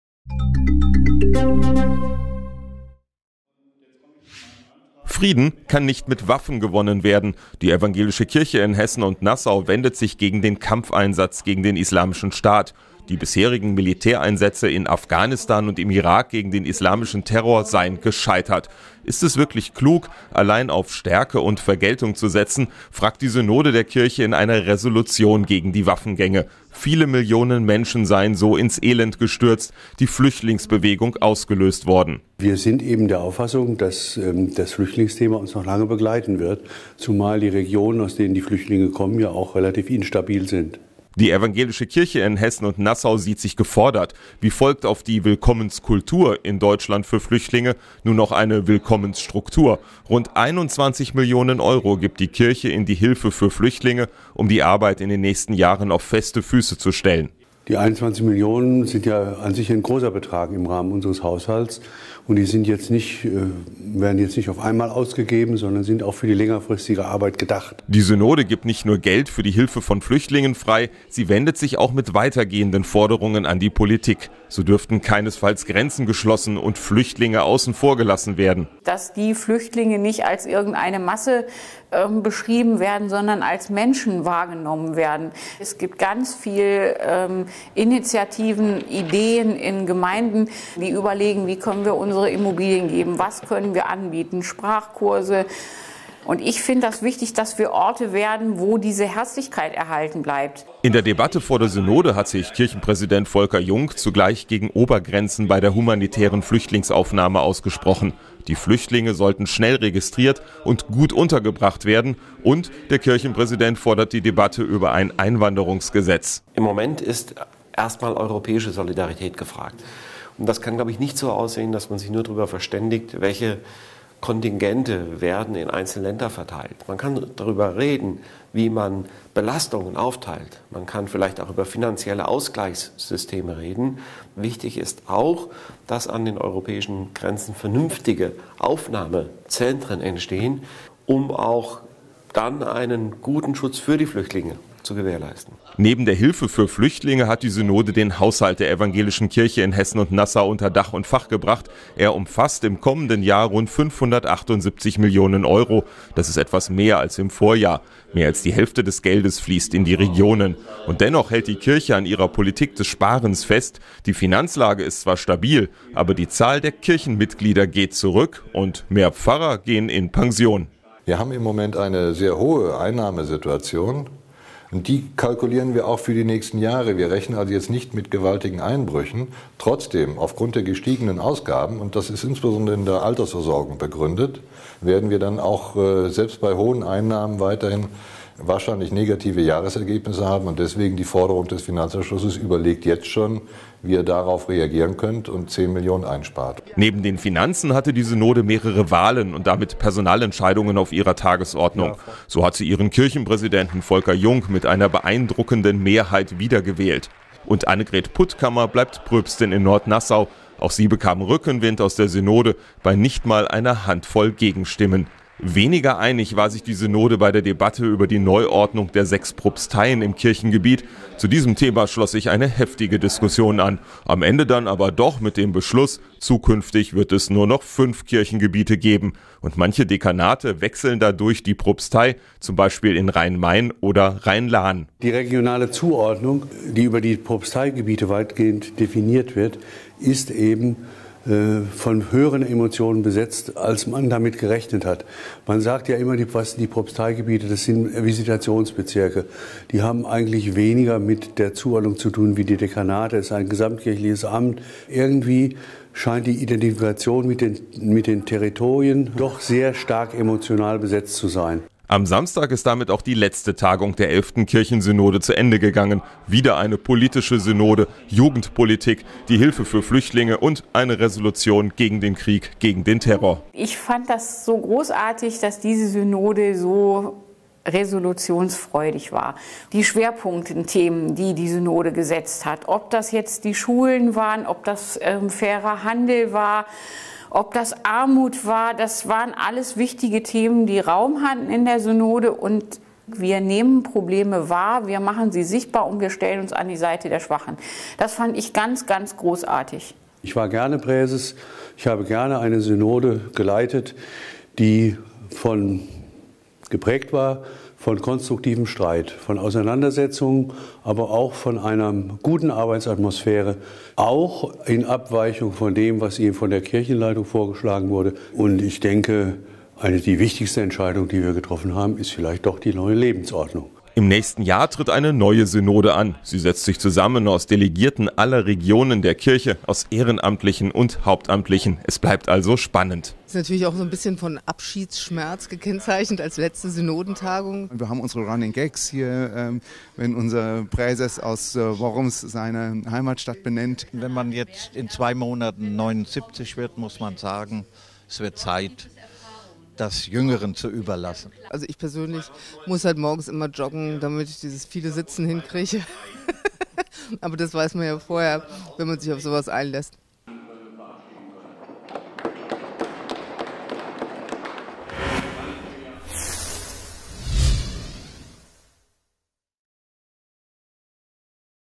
EKHN Herbstsynode 2015 ist zu Ende gegangen
Die Herbstsynode der EKHN ist zu Ende gegangen. Neben der Wiederwahl von Kirchenpräsident Volker Jung stand auch der Haushalt von 2016 auf der Agenda. Knapp 21 Millionen Euro will die EKHN für die Flüchtlingshilfe ausgeben.